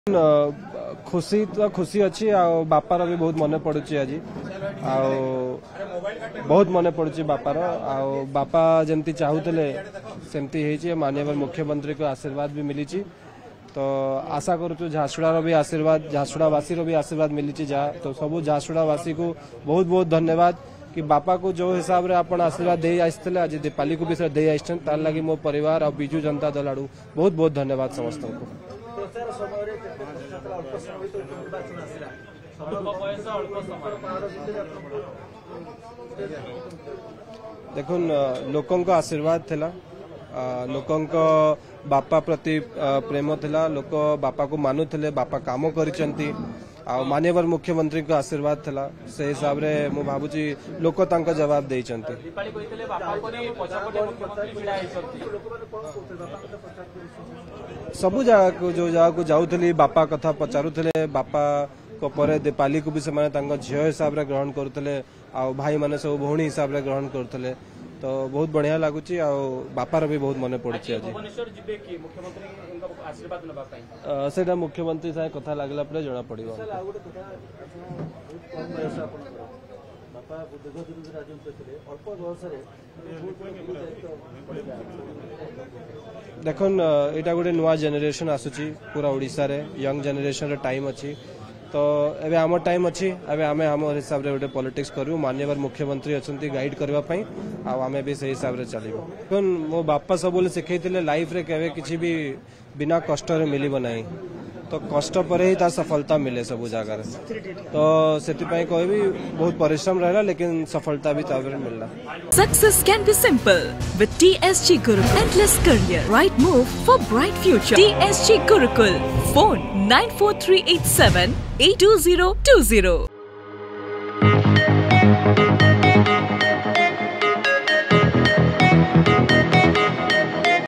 ‘କଷ୍ଟ ପରେ ସଫଳତା ମିଳେ’ | Odisha Reporter
ବାପାଙ୍କ ଭଳି ଦୀପାଳିଙ୍କୁ ମଧ୍ୟ ଭଲ ପାଇବା ଦେଲେ ଝାରସୁଗୁଡା ବାସୀ। ଆମକୁ କଷ୍ଟ ପରେ ସଫଳାତା ମିିଳିଲା ବୋଲି କହିଲେ ଦୀପାଳି ଦାସଙ୍କ ଭାଇ ।